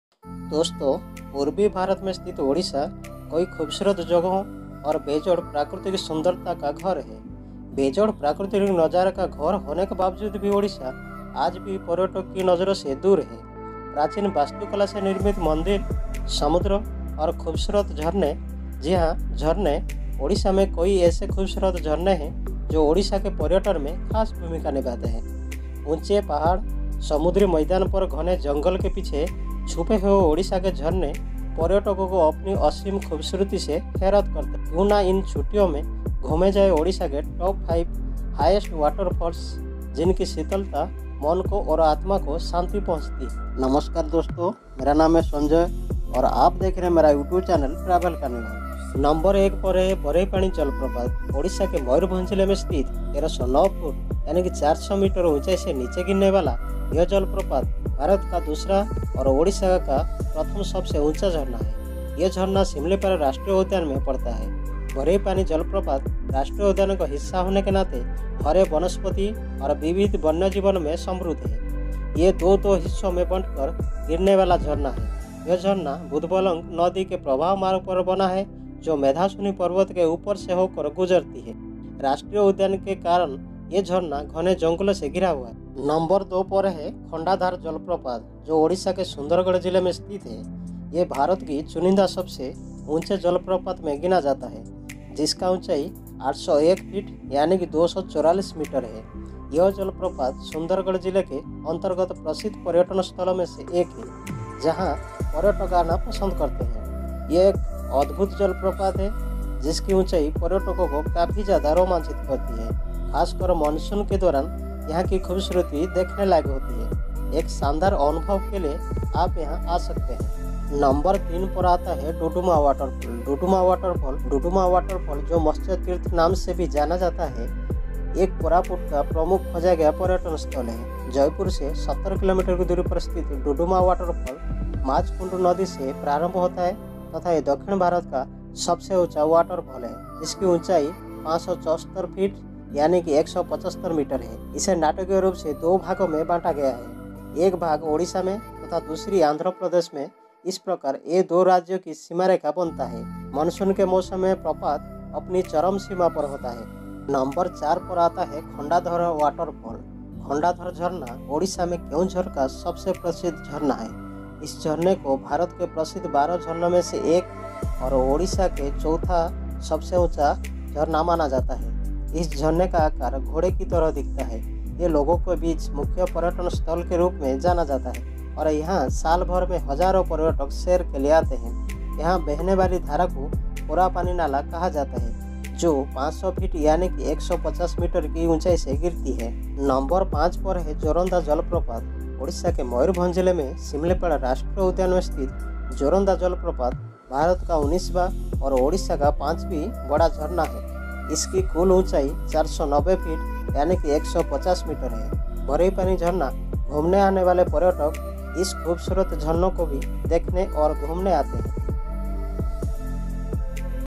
Top 5 highest Waterfalls in Odisha // ओड़िशा कि 5 सबसे ऊँची खूबसूरत जलप्रपात
This video is information about the top 5 highest waterfalls in odisha.
1:)
2:)
3:)
4:)
5:)

Travel Carnival